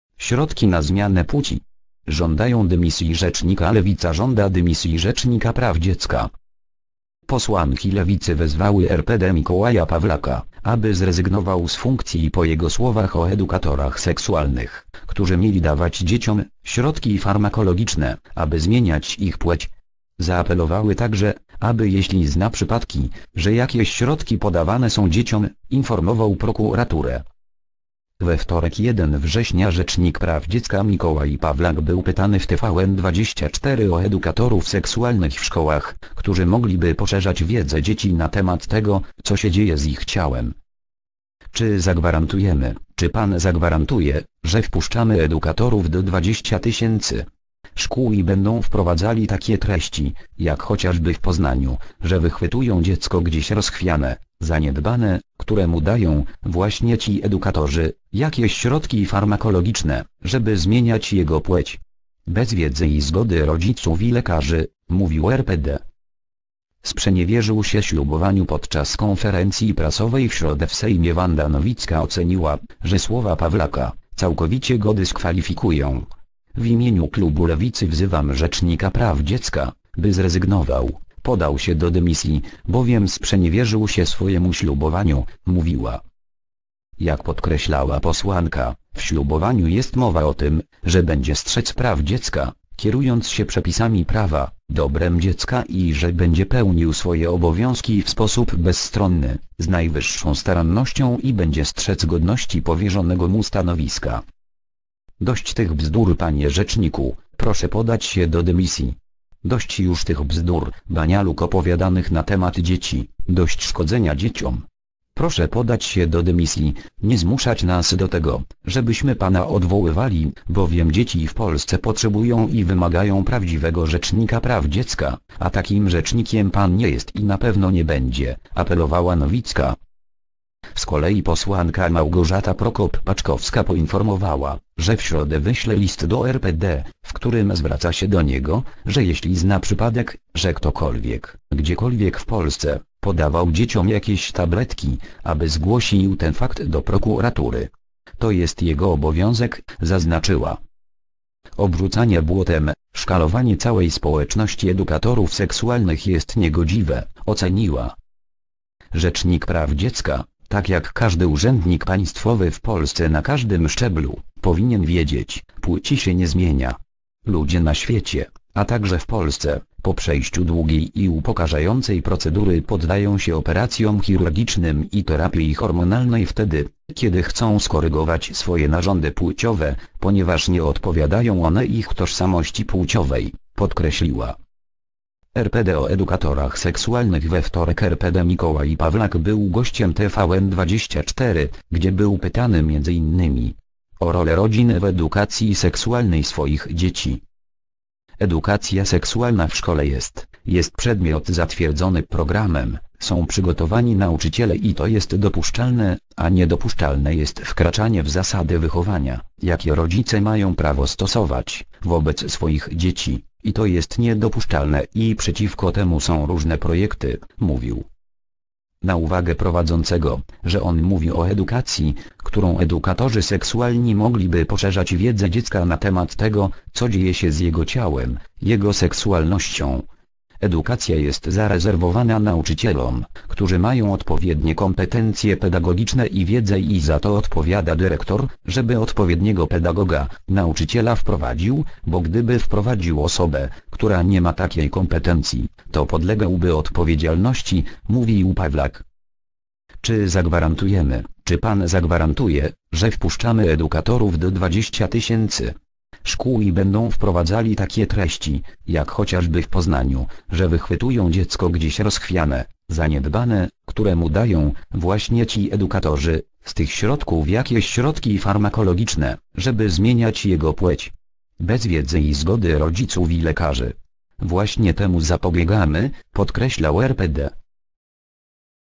"Środki na zmianę płci". Żądają dymisji rzecznika.
Lewica żąda dymisji Rzecznika Praw Dziecka.

  (fot. arch. PAP/Tomasz Gzell)